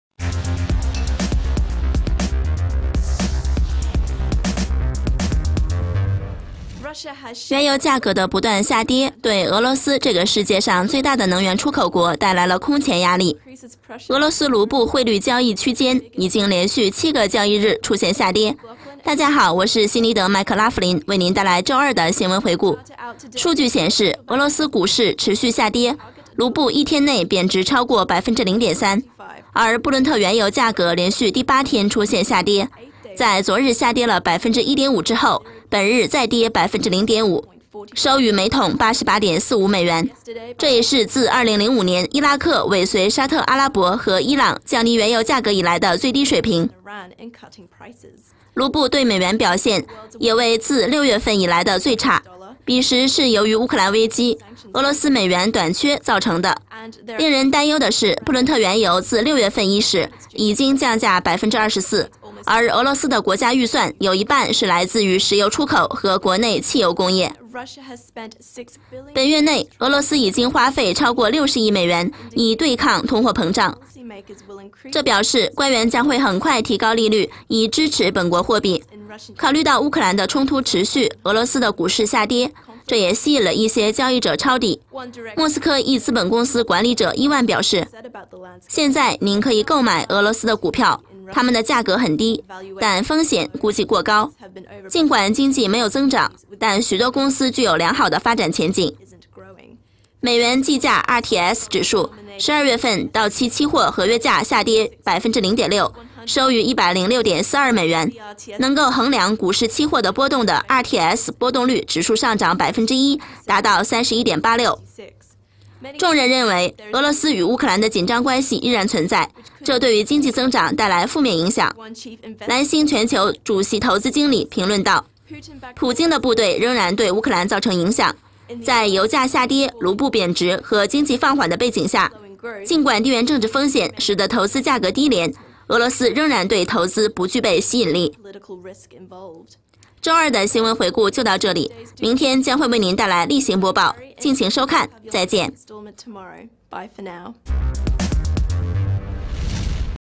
俄罗斯滑坡 - 14.10.2014 - Dukascopy Press Review
原油价格的不断下跌对俄罗斯这个世界上最大的能源出口国带来空前压力。